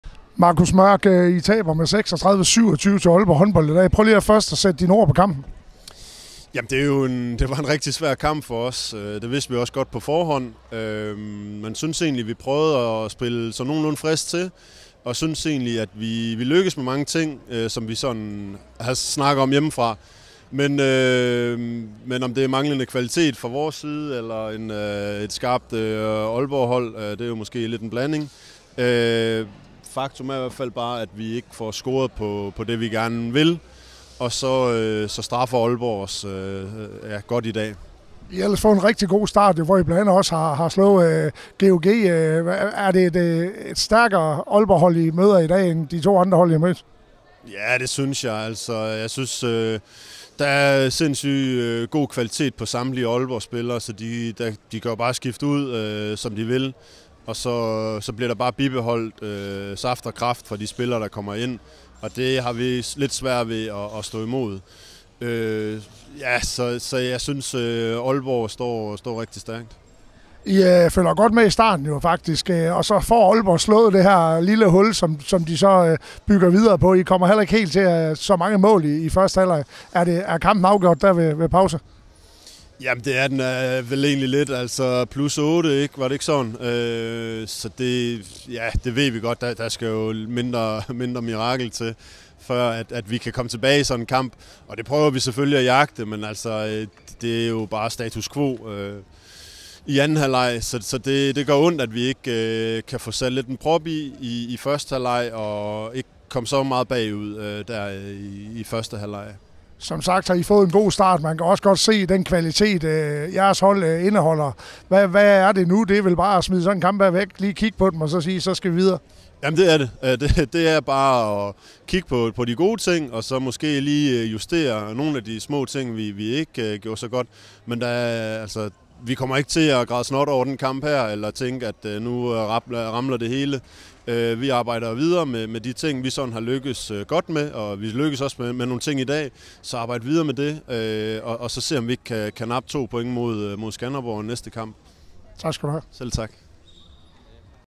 Aalborg Håndbold-Ribe/Esbjerg HH, 10/9-2023.
Rishøjs Corner fangede Ribe/Esbjergs Marcus Mørk efter nederlaget til Aalborg Håndbold på udebane.